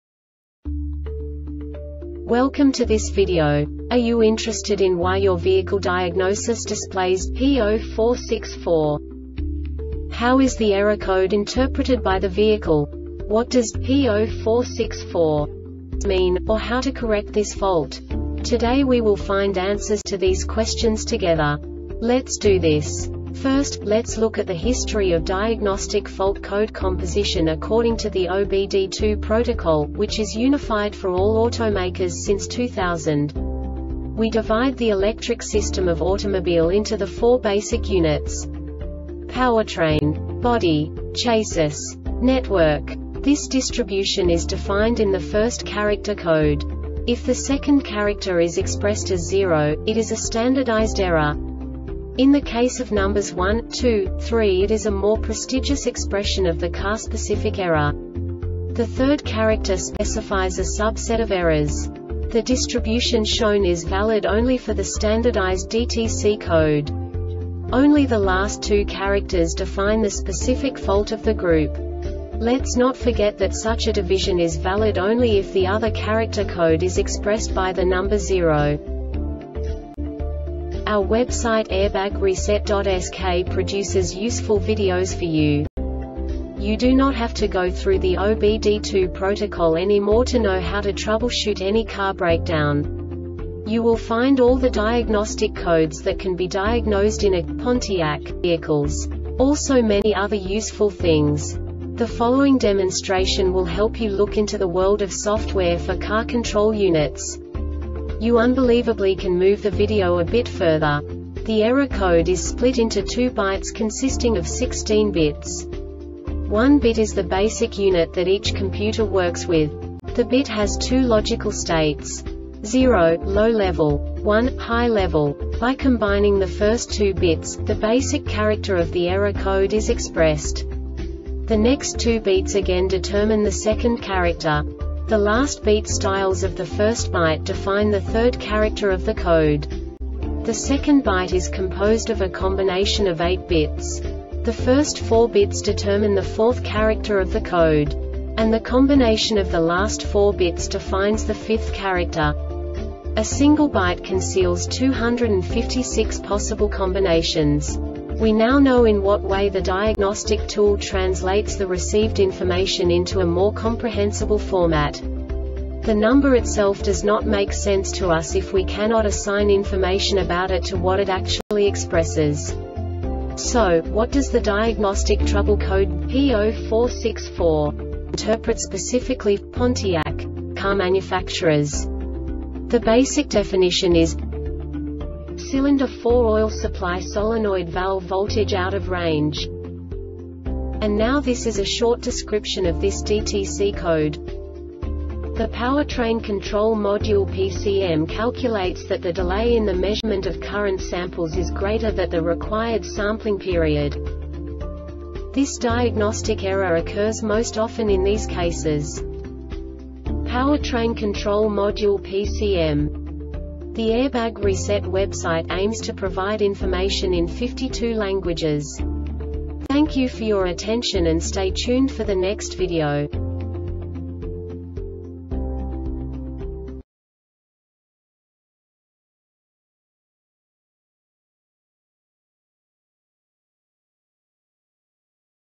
DTC Pontiac P0464 Short Explanation
Contents:
0:21 Basic DTC analysis according to OBD2 protocol standard.
1:48 Insight into programming
2:58 A diagnostically understandable description of the Diagnostic Trouble Code
3:05 Basic error definition

Make:
Pontiac
Code:
P0464
Definition:
Cylinder 4 Oil Supply Solenoid Valve Voltage Out Of Range
Description:
The Powertrain Control Module (PCM) calculates that the delay in the measurement of current samples is greater that the required sampling period.